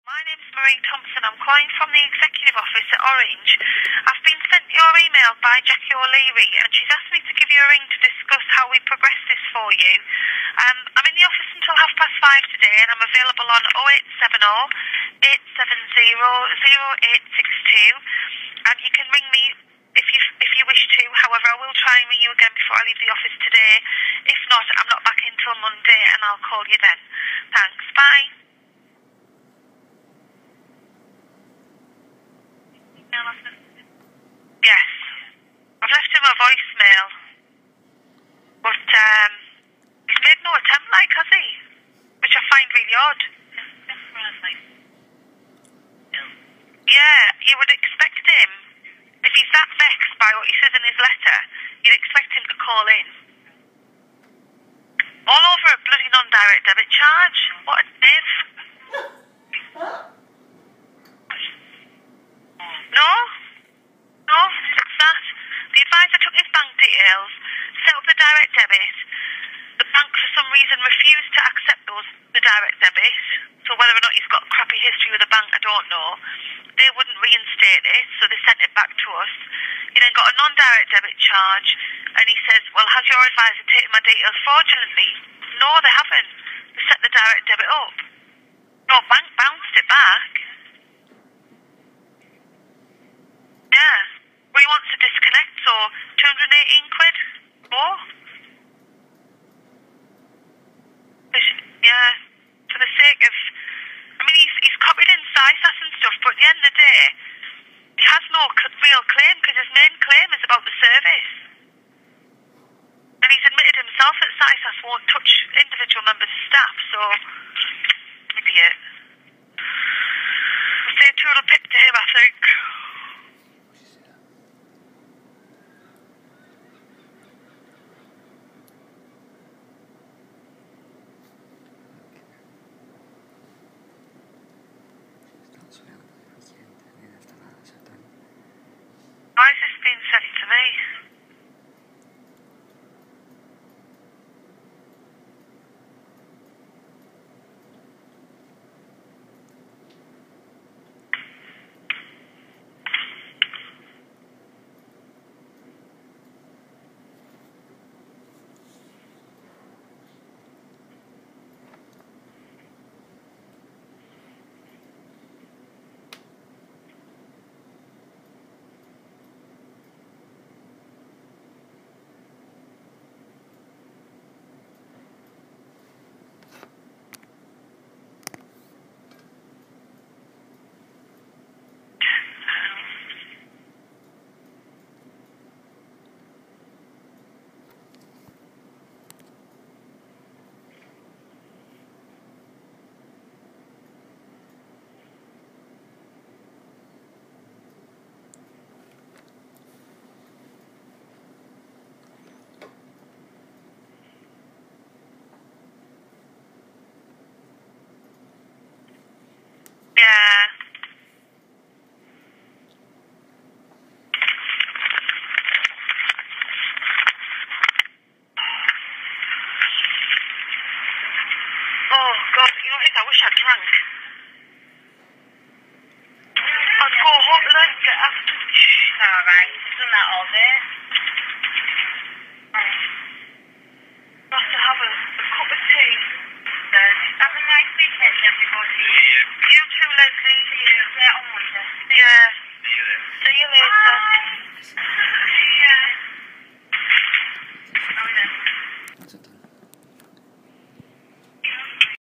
Voicemail left by Orange
This is a voicemail that was left on my answering machine when I sent an e-mail to complain about Orange and it's service, customer service and billing. Needless to say, Orange agreed to cancelling my contract before it was due for renewal, with no cancelation fee being payable.